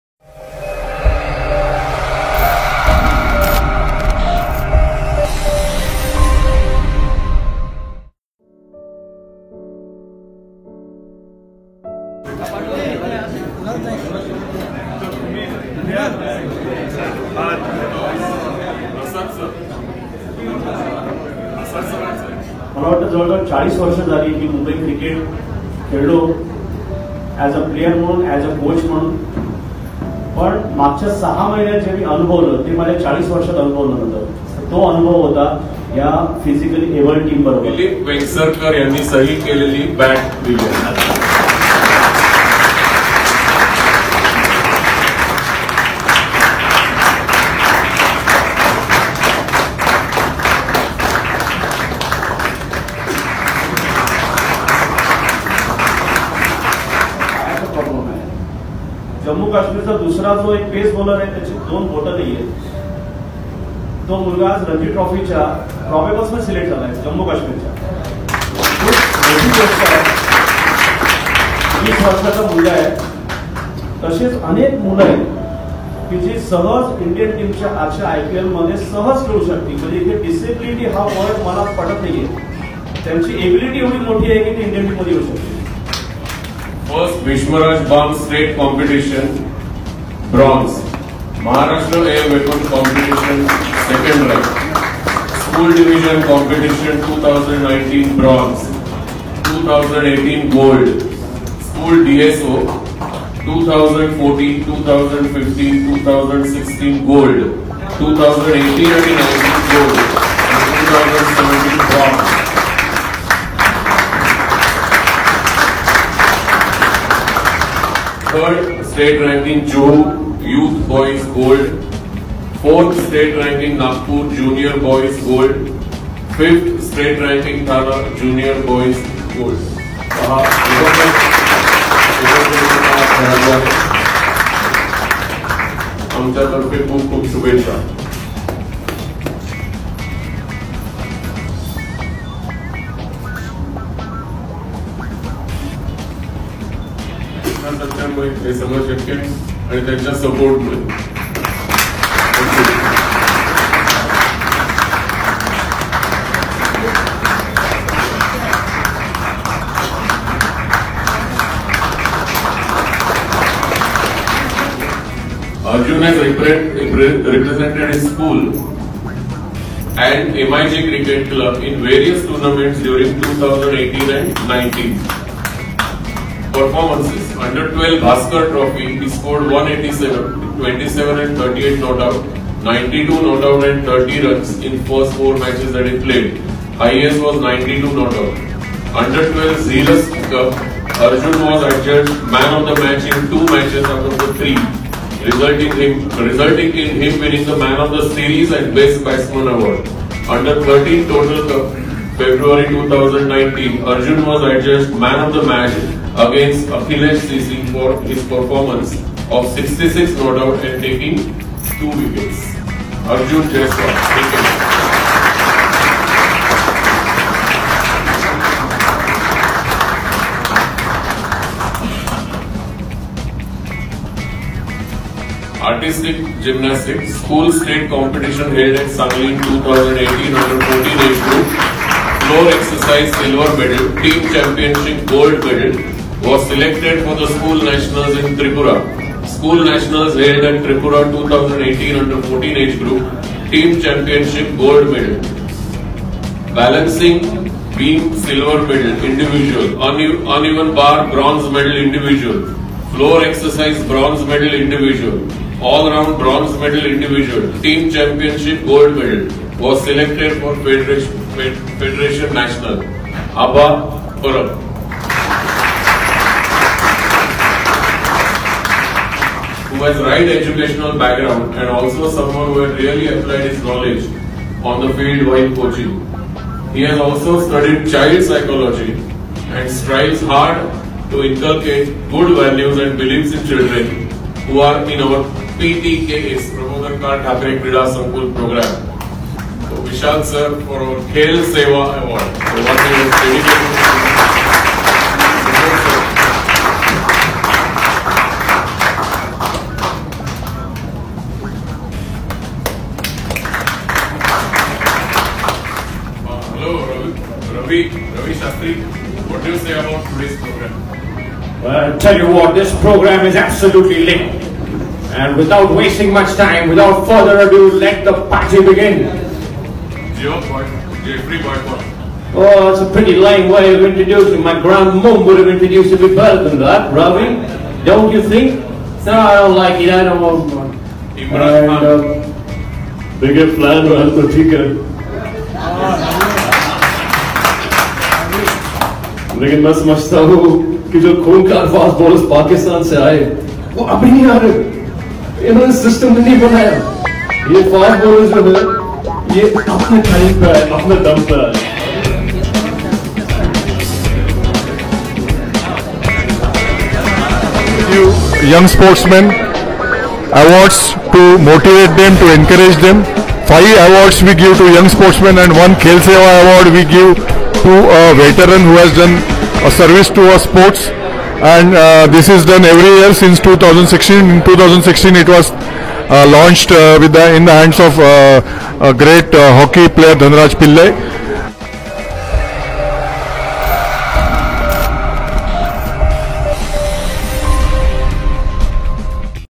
Sports Awards 2019 by Sportsnasha
Sports Awards 2019 by Sportsnasha is a video prepared by Taus Rizvi of  passion sports for sportsnasha. The video shows glimpses of young sportsman award ceremony.